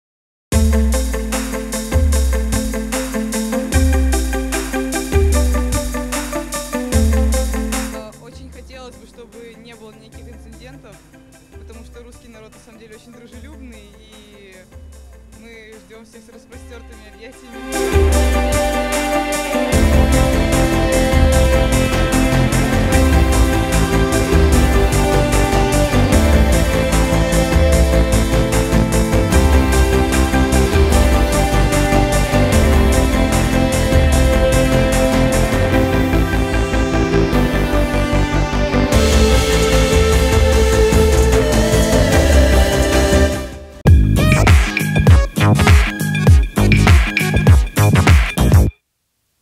A 100 días para el Mundial Rusia 2018
El presidente ruso, Vladímir Putin, demuestra sus habilidades con el balón haciendo unos pases en el Kremlin con el presidente de la FIFA, Gianni Infantino, en un video promocional del organismo a cien días del comienzo del Mundial de Fútbol 2018 en